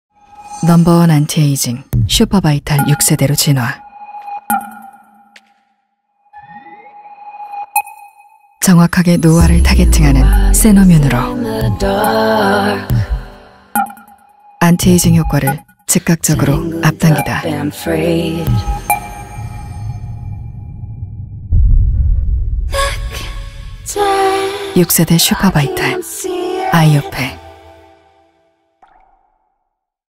[아이오페] 안티에이징 효과를 즉각적으로 앞당기다, 아이오페 슈퍼바이탈 6세대 30s
정확하게 노화를 타겟팅하는 최초*의 세노뮨™으로
안티에이징 효과를 앞당기다

18년 동안 안티에이징의 패러다임을 바꿔온
No.1** 안티에이징 슈퍼바이탈 6세대 진화

단 한 번 사용으로 모공 탄력, 잡티, 투명도, 잔주름 등
10대 노화 지표 가 개선되는 슈퍼바이탈 크림의
즉각적인 안티에이징 효과***를 지금 바로 경험해보세요.

피지선 활동이 급격히 저하된  건조 노화 피부를 위해
섬세하고 정밀하게 설계된 슈퍼바이탈 오일 블렌딩 세럼의
항산화 안티에이징 시너지도 지금 바로 경험해보세요.

*AP최초